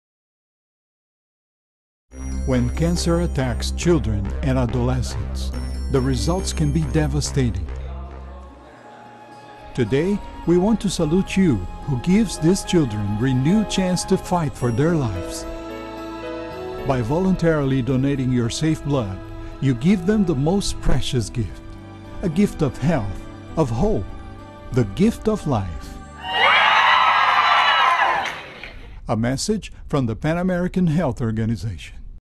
Blood Donors
A Public Service Announcement produced by PAHO to honor blood donors who help to save the lives of children around the Americas and the Caribbean.